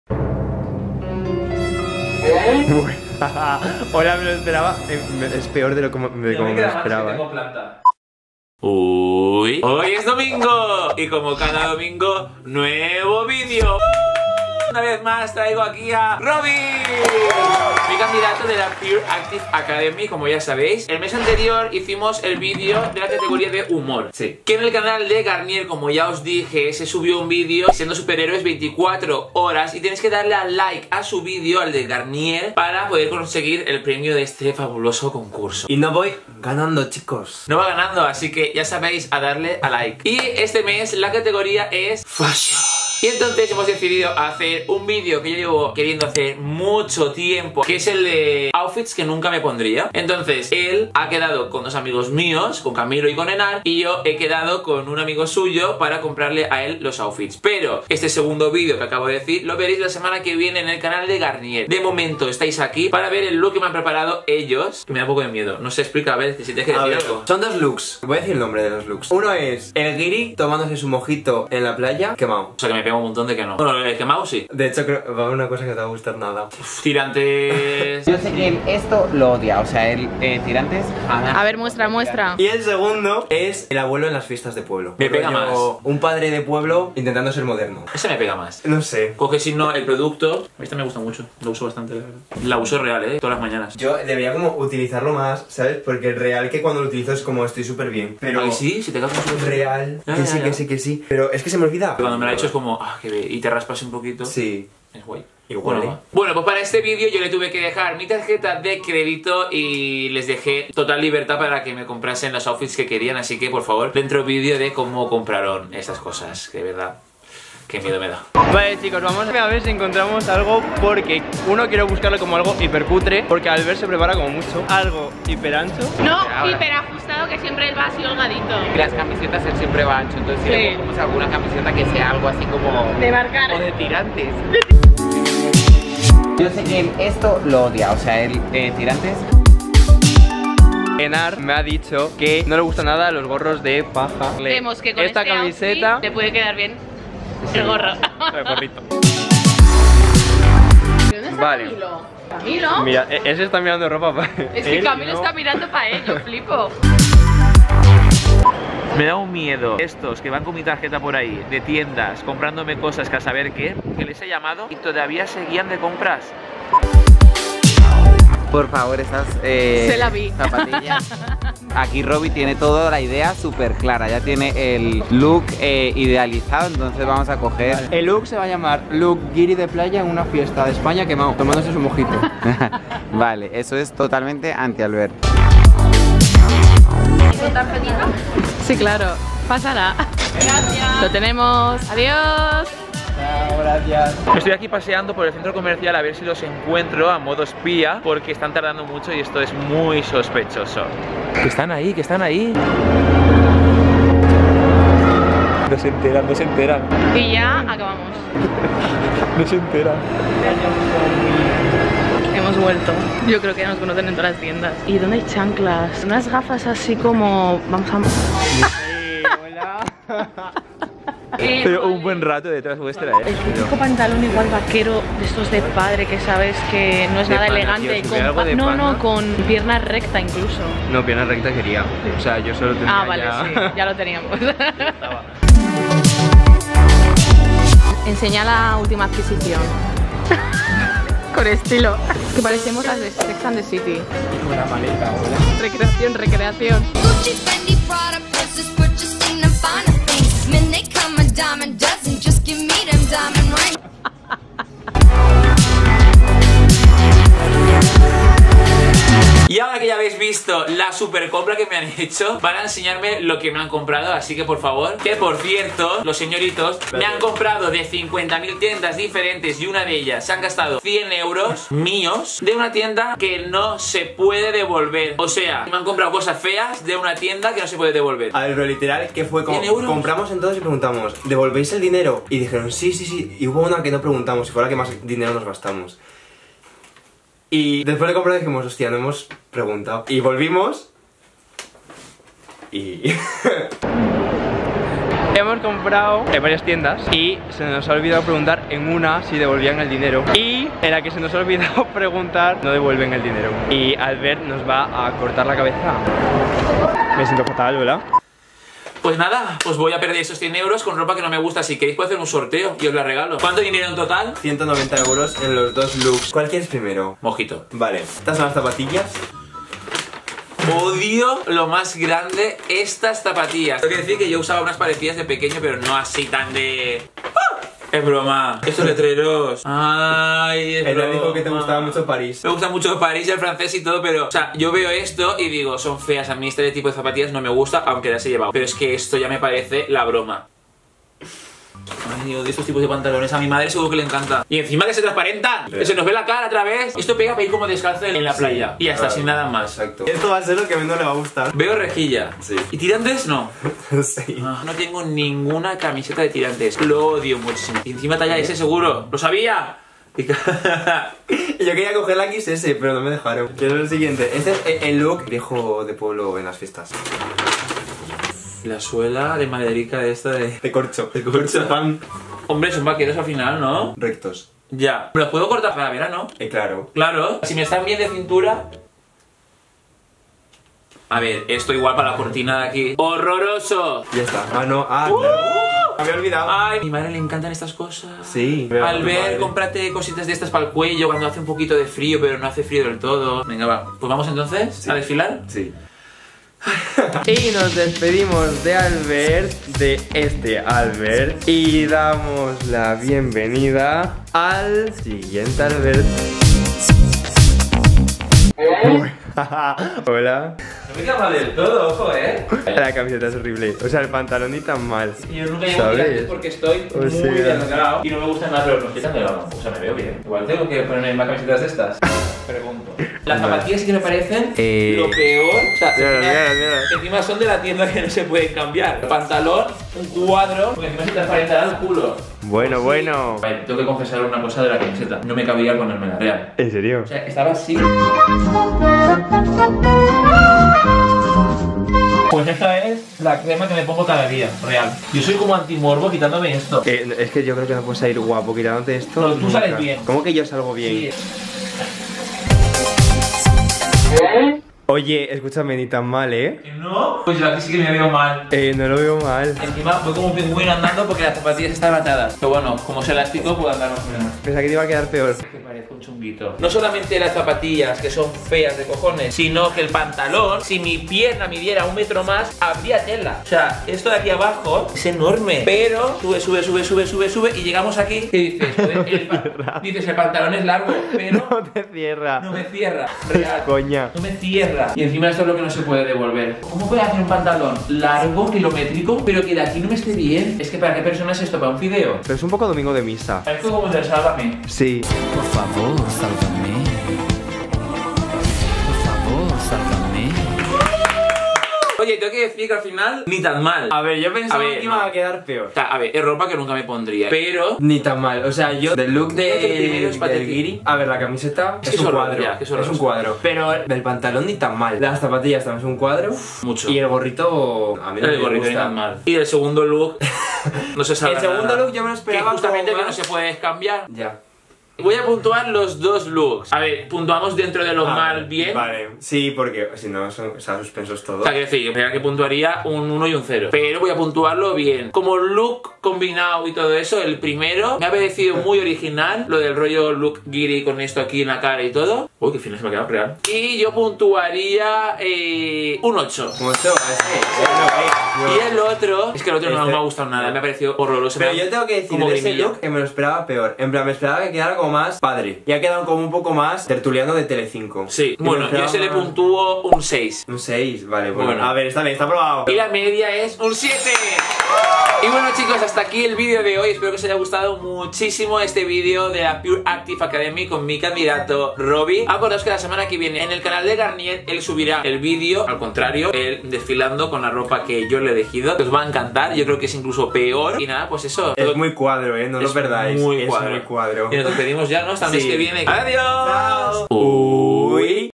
OUTFITS QUE NUNCA ME PONDRIA *CAMBIO DE LOOK* | Uy Albert!
Como sabéis @arovinar es mi elegido de la Pure Active Academy y yo soy su coach! Este esnuestro segundo Coach Session con la categoria FASHION.